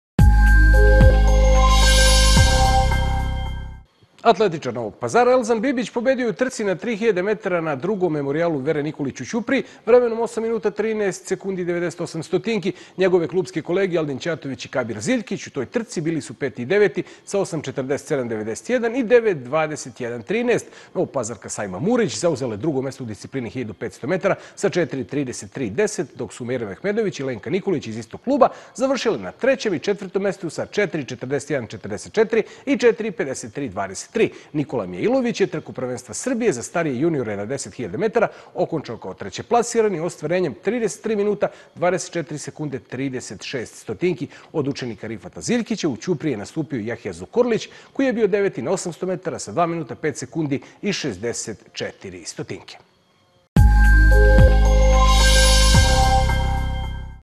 Memorijal Vere Nikolić u Ćupriji: Elzan Bibić najbolji na 3 000 metara
Atletičar Novog Pazara Elzan Bibić pobedio je u trci na 3.000 metara na drugom Memorijalu Vere Nikolić u Ćupriji vremenom od 8:13,98 minuta. Njegove klupske kolege Aldin Ćatović i Kabir Ziljkić u toj trci bili peti i deveti sa 8:47,91 i 9:21,13 minuta. Novopazarka Saima Murić zauzela je drugo mesto u disciplini 1.500 metara sa 4:33,10, dok su Mejra Mehmedović i Lenka Nikolić iz istog kluba završile na trećem i četvrtom mestu sa 4:41,44 i 4:53,23 minuta. Nikola Mijailović je trku Prvenstva Srbije za starije juniore na 10.000 metara okončao kao trećeplasirani ostvarenjem od 33:24,36 minuta. Od učenika Rifata Ziljkića, u Ćupriji je nastupio i Jahja Zukorlić, koji je bio deveti na 800 metara sa 2:05,64 minuta.